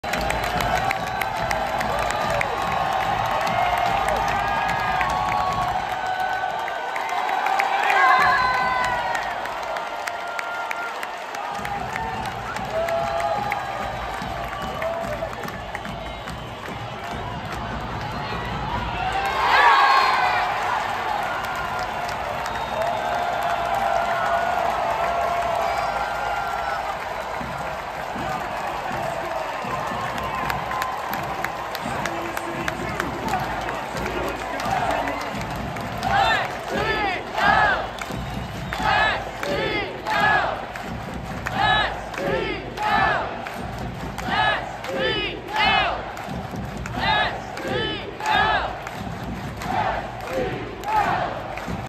End of St. Louis City SC vs LA Galaxy at CITYPARK (9/1/24)
Final Score
STL 2-1 LA